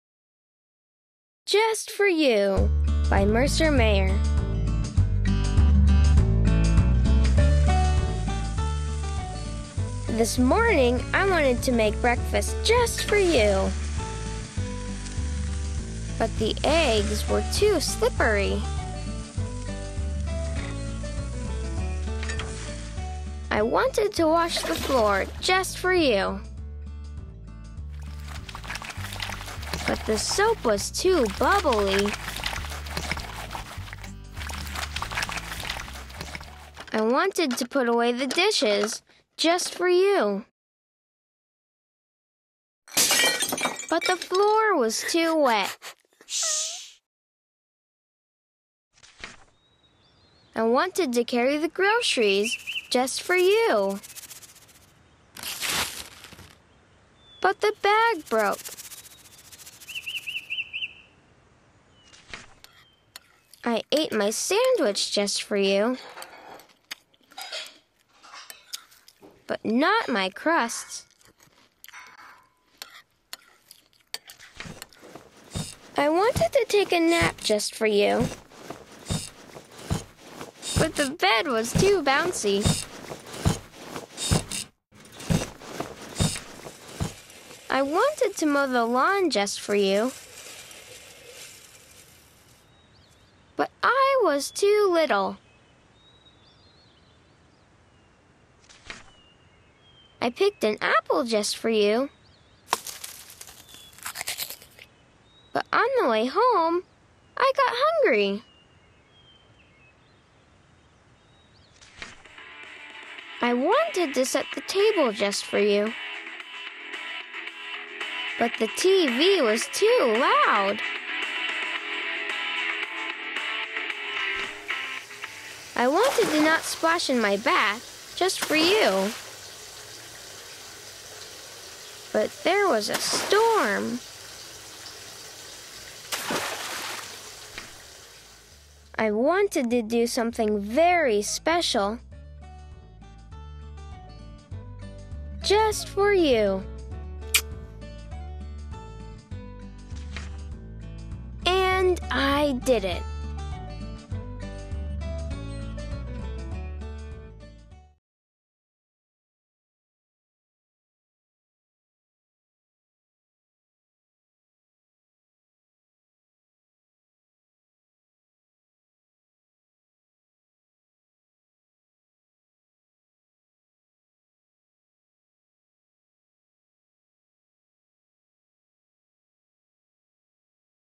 Just For You by Mercer Mayer - Little Critter - Read Aloud Books for Children - Storytime
Sweet Little Critter wants to do something special for his mom.

Sweet Little Critter wants to do something special for his mom. He tries doing big kid chores, but he's too little to get them quite right! What unique gift will Little Critter find to give to his mom?